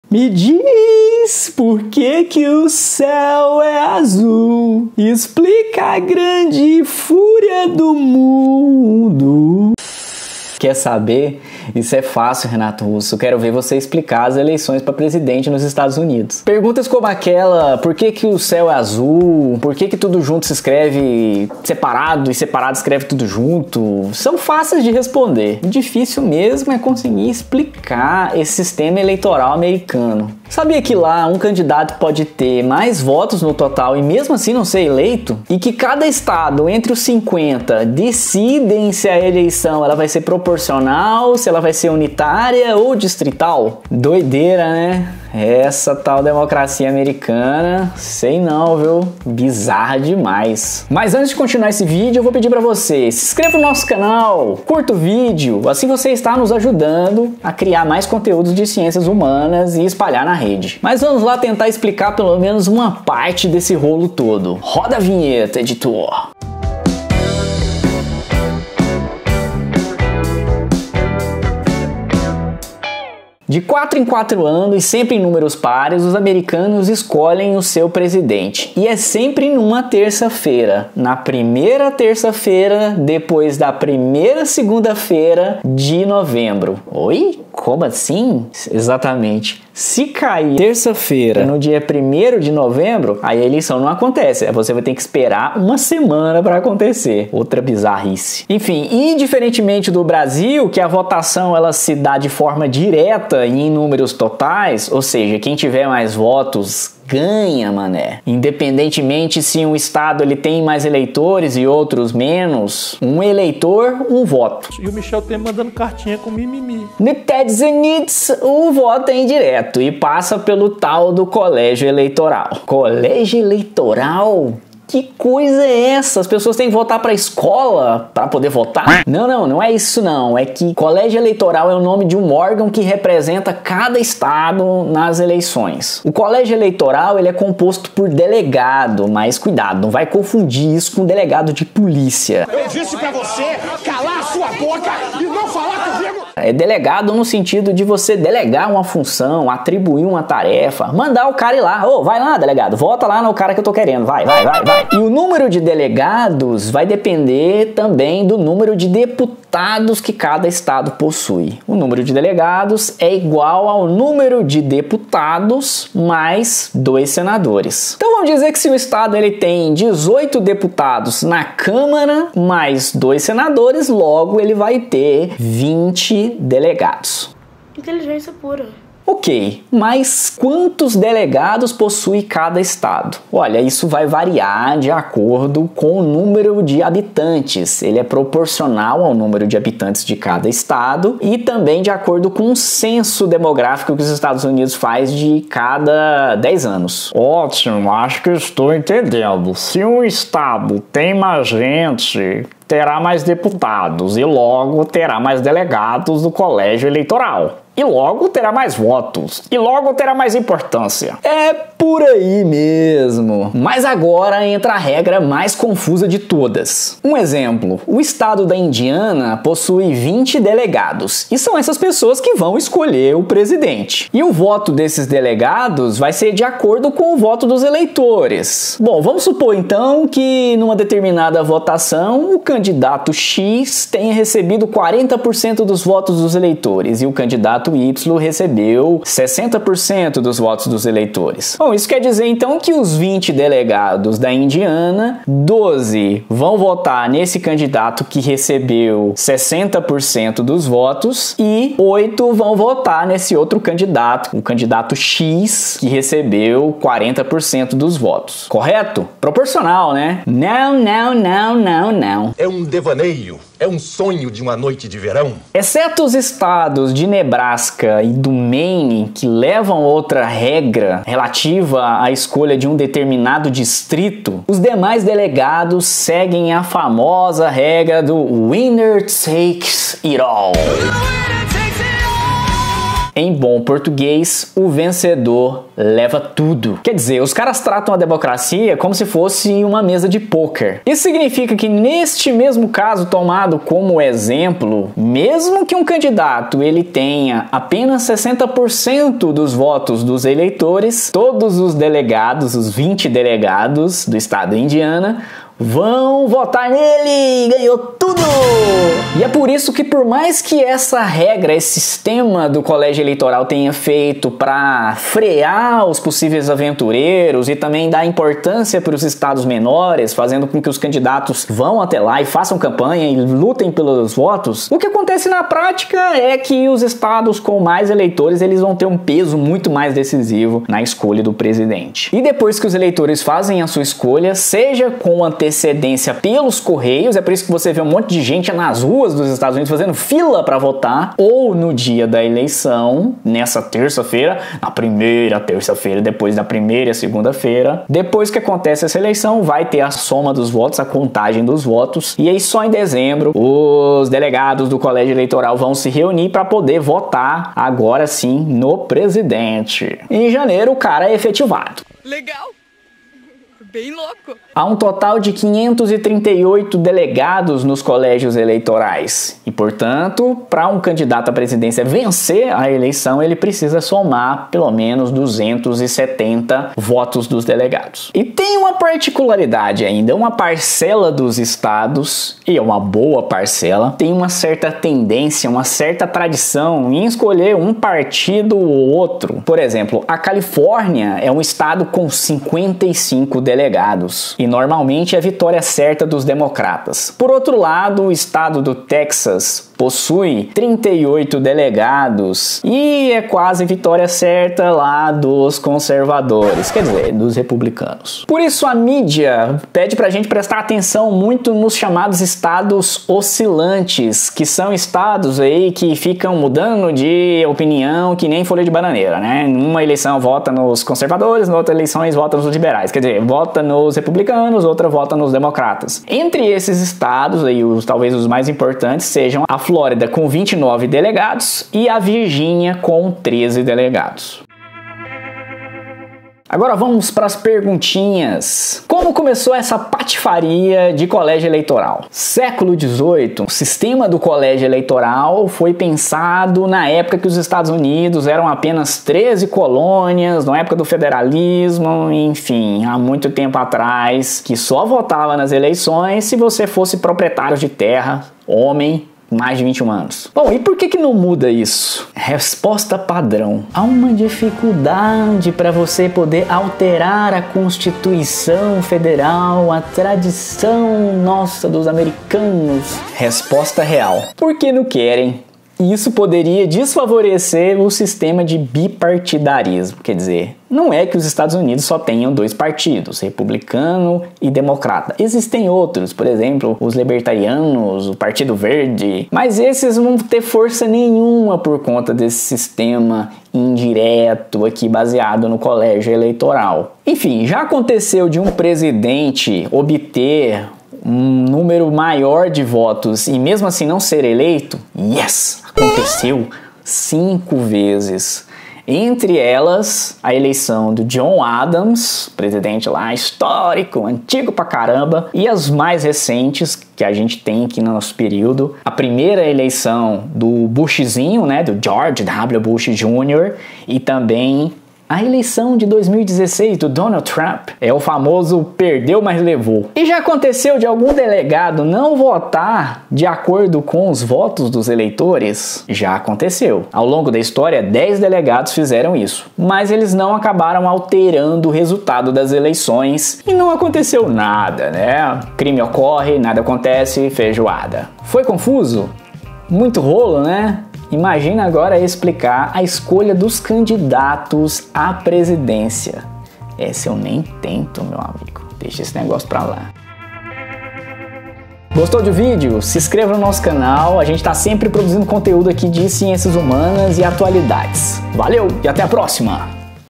Como é decidido quem será presidente dos Estados Unidos?
O vídeo acima, o Canal Diacrônico traz um resumo sobre o funcionamento do sistema eleitoral dos Estados Unidos na escolha do presidente.

Apresentação e edição: Munís Pedro Alves, professor de história e ciências sociais no Ensino Médio

Comentários ofensivos e discriminatórios serão excluídos. Críticas e sugestões são bem-vindas.